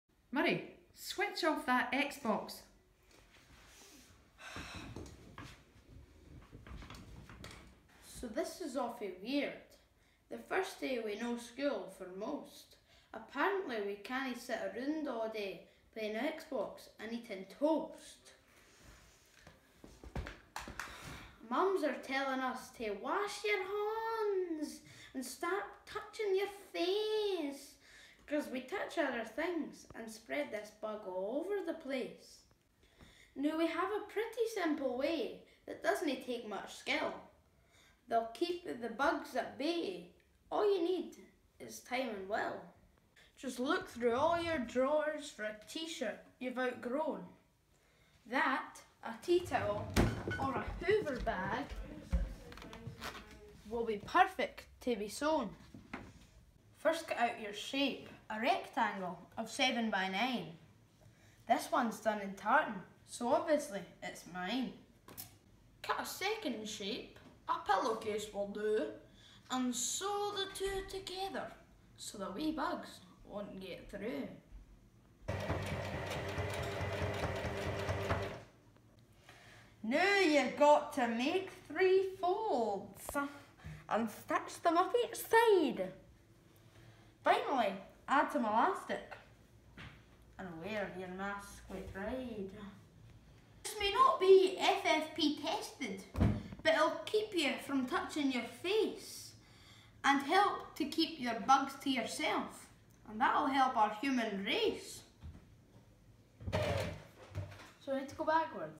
Avoiding Corona
Advice on how to make a mask so that we stop spreading germs.